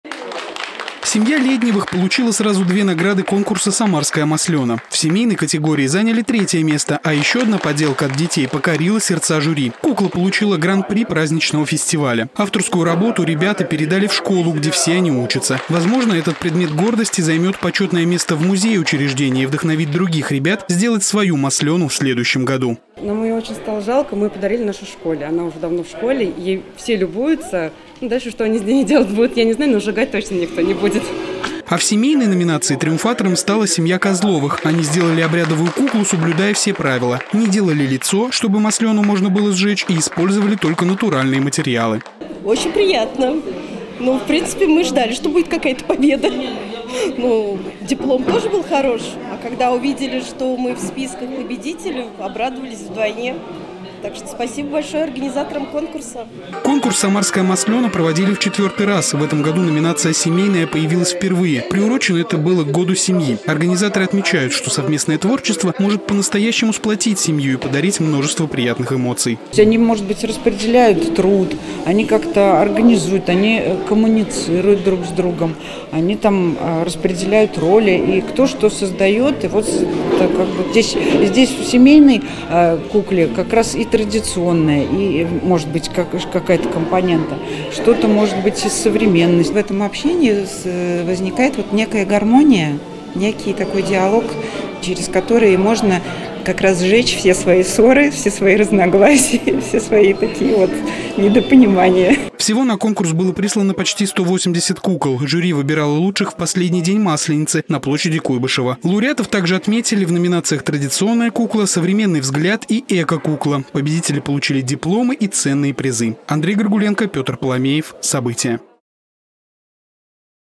В Самаре определили лауреатов конкурса "Самарская маслена"
Показали творческий подход к созданию обрядовых кукол. В библиотеке народов Поволжья прошло награждение победителей конкурса "Самарская маслёна". Его провели в 4-й раз. Жюри оценило более 170 кукол, символизирующих зиму, которые принято сжигать в последний день масленицы, провожая холодное время года. С лауреатами и организаторами конкурса пообщалась наша съёмочная группа.